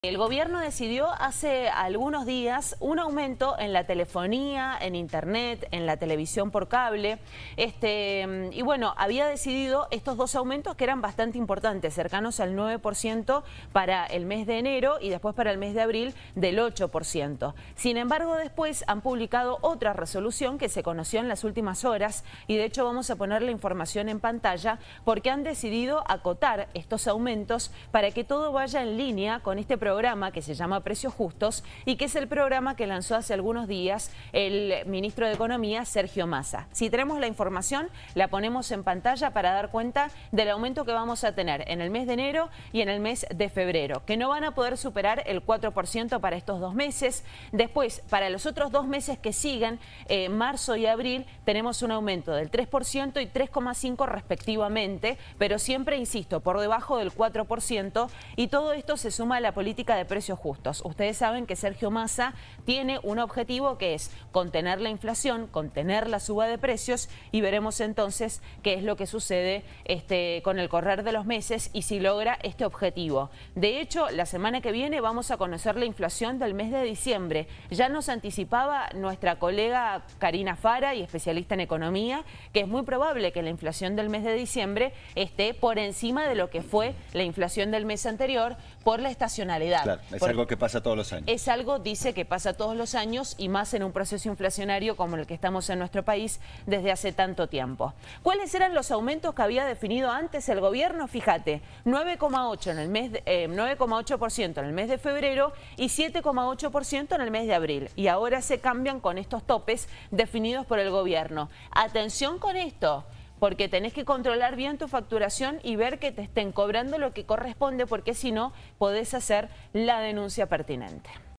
AUMENTOS EN TELEFONÍA, INTERNET Y TV POR CABLE
El gobierno confirmó un aumento de hasta un 4% mensual en los servicios de televisión paga, telefonía e internet. Los incrementos de enero a abril se llevarán a cabo de la siguiente forma: 4% en enero, 4% en febrero, marzo un 3% y abril 3,5%, dando un total de 15% en los primeros cuatro meses del año.